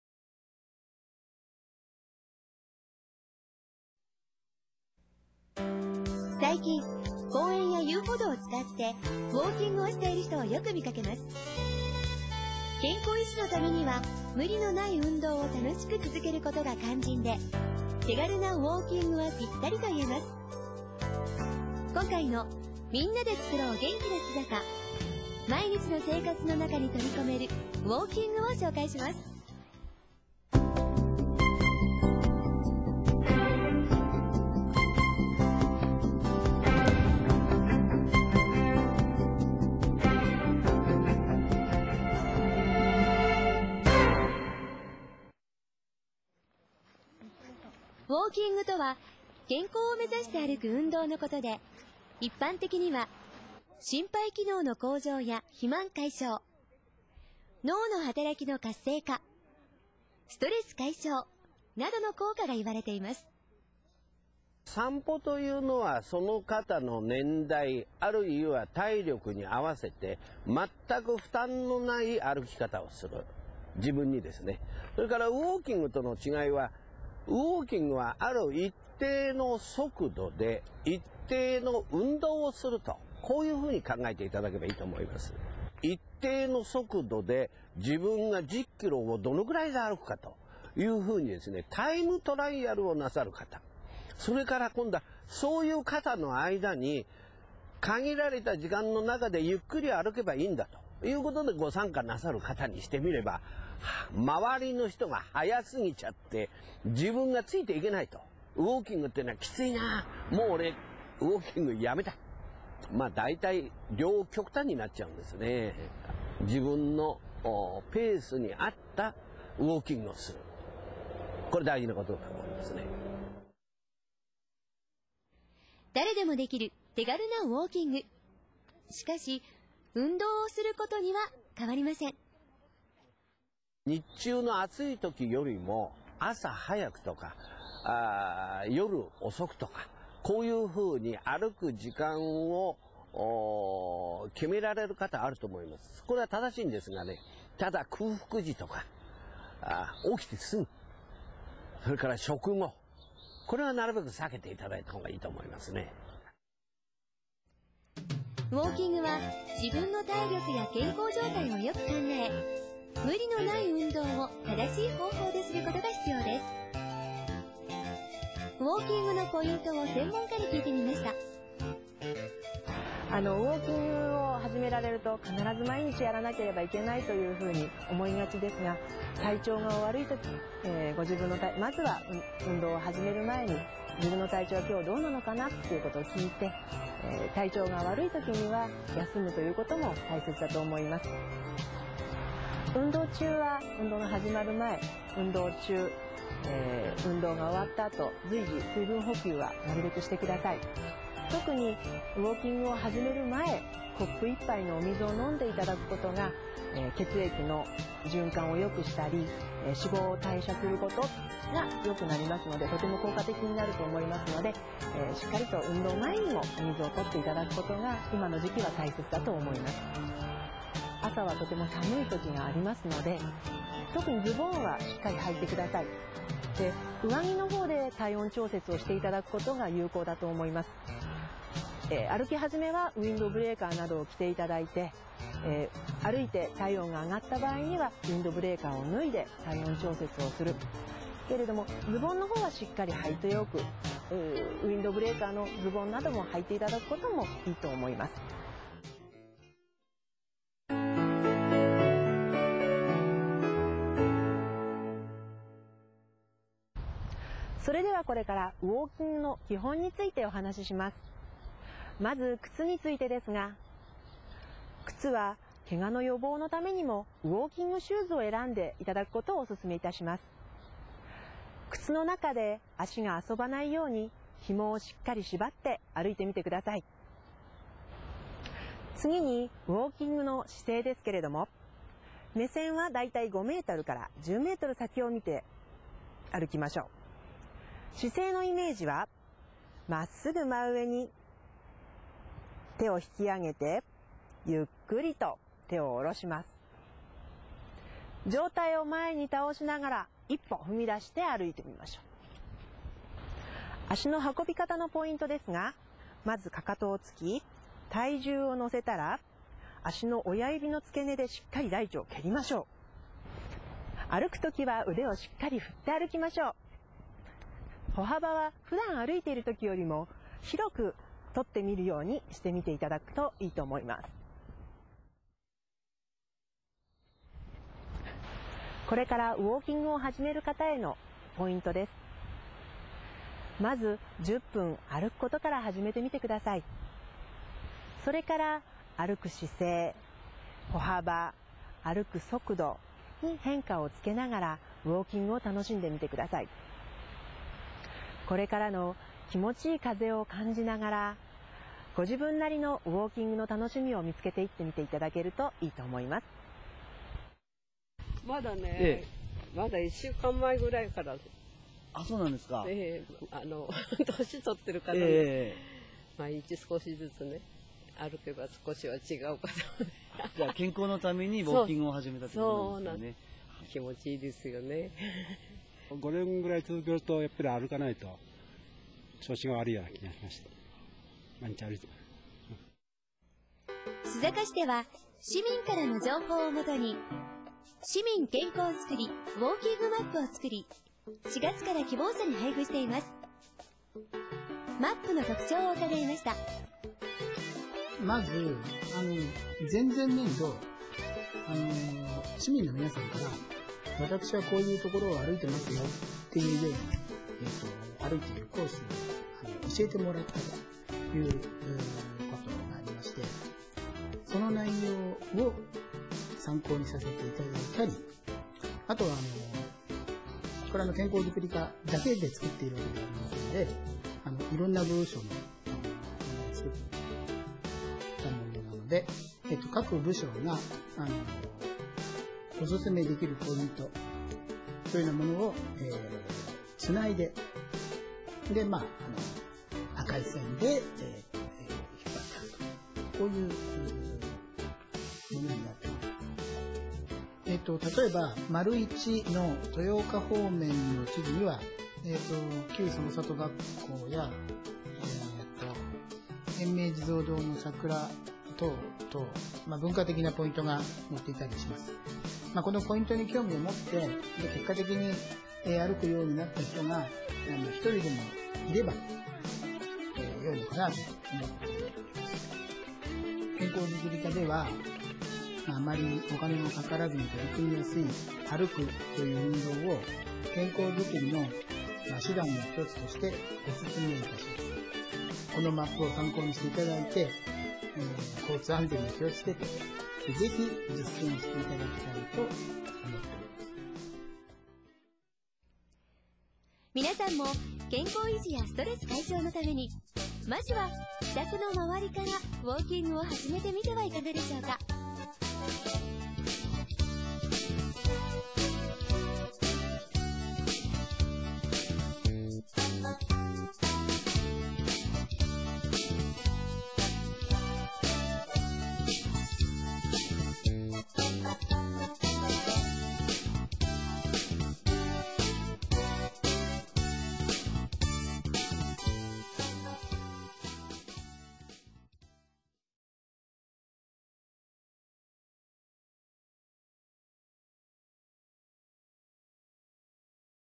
"共創" 皆で つくろう 元気な すざか ～ウォーキングで健康になろう～2
須坂市は、市民と行政の「共創」のまちづくりを進めています。
「共創」とは、分野の異なる（独創性のある）人々がお互いにアイデアを提供し、
横の連携をし、創造していくことです。
須高ケーブルテレビでは、「共創」の理念のもと、活動しているボランティアなどを
この番組で紹介しています。